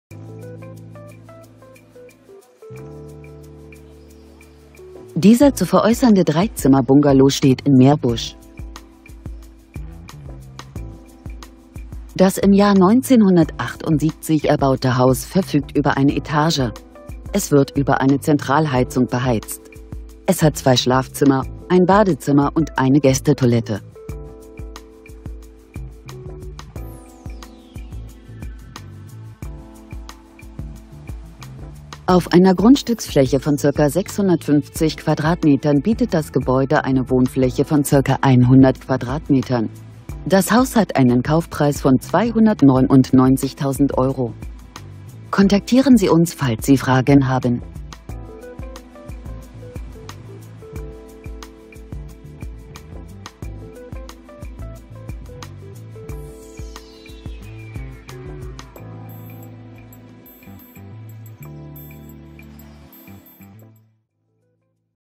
Bequem, ruhig und naturnah - so wollen wir leben!
Bungalow
Preis: 299.000€
Zimmer: 3
Wohnfläche: ca. 100m²

Joachim Bohn

Heinrich-Hertz-Straße
40699 Erkrath